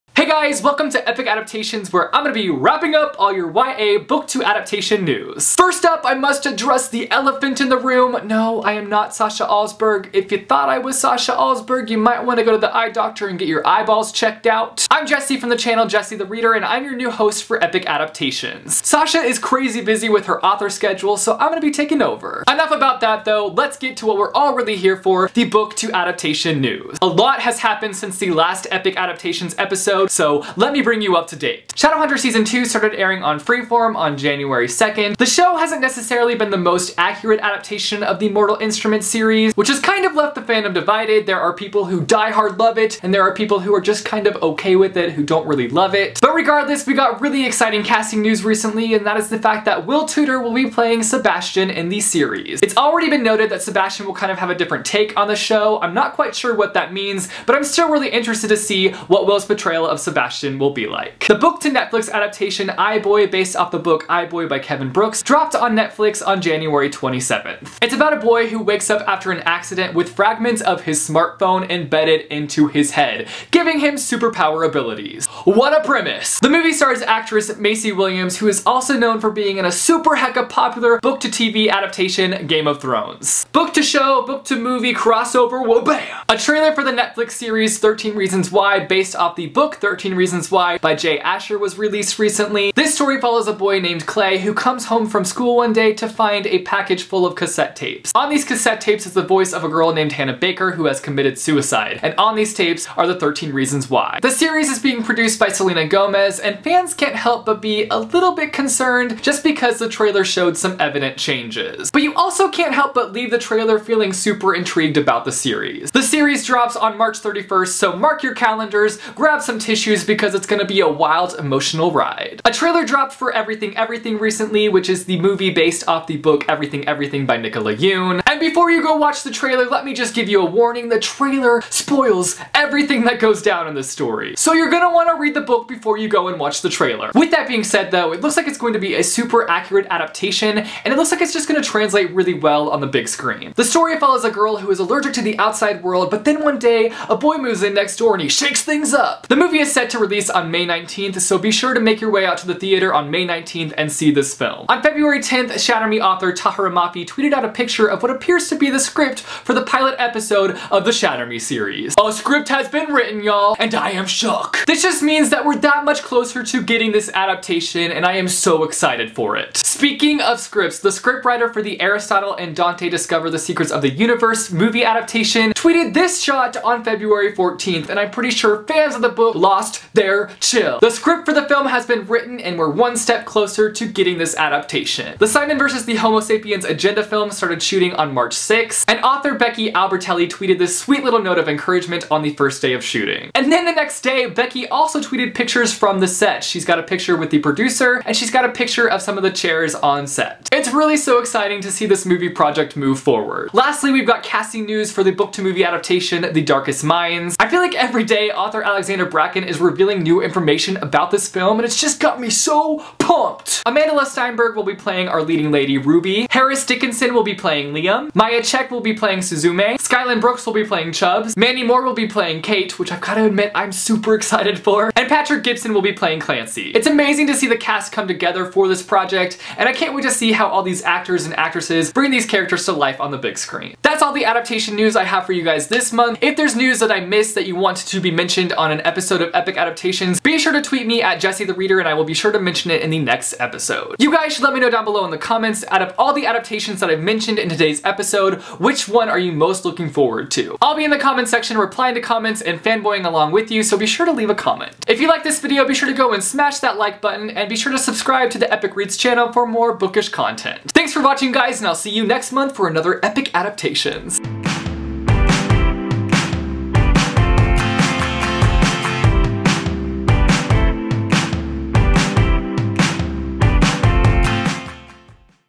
13 Reasons Why & Shatter Me Script ft. Jessethereader | Epic Adaptations
Jesse from jessethereader brings us all your YA book-to-movie news in Epic Adaptations! Watch for the most up-to-date news on all your favorite adaptations, including Shadowhunters: The Mortal Instruments, as well as Netflix originals iBoy and 13 Reasons Why. Also discussed, Becky Albertalli on the set of Simon vs. the Homo Sapiens Agenda, a trailer for Everything, Everything, a script for Shatter Me, Darkest Minds casting news, and so much more!

Books featured in this EpicAdaptations
The Mortal Instruments Series by Cassandra Clare
Thirteen Reasons Why by Jay Asher
iBoy by Kevin Brooks
Everything, Everything by Nicola Yoon
Shatter Me by Tahereh Mafi
Aristotle and Dante Discover the Secrets of the Universe by Benjamin Alire Sáenz
Simon vs. the Homo Sapiens Agenda by Becky Albertalli
The Darkest Minds by Alexandra Bracken